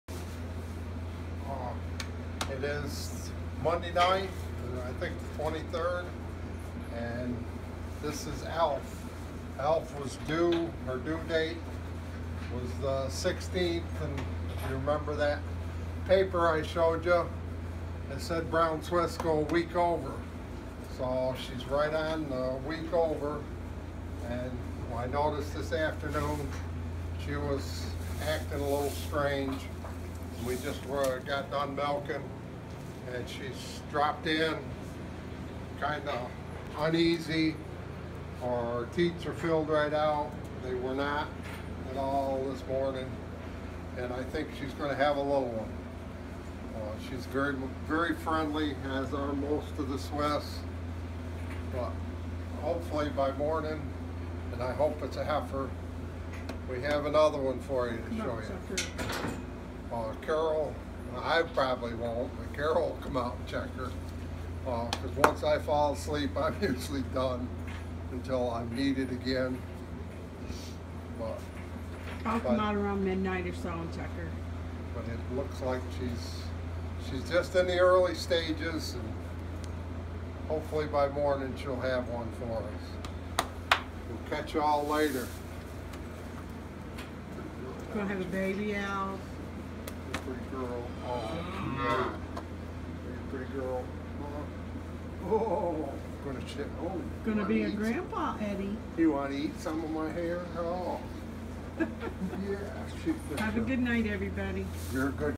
Baby watch 3--ELF
We put Elf in the barn tonite. Looks like we will have a new baby tonite. This will be #3 born in this pen during a storm. SNOW SLEET AND FREEZING RAIN tonite. Oh boy!!!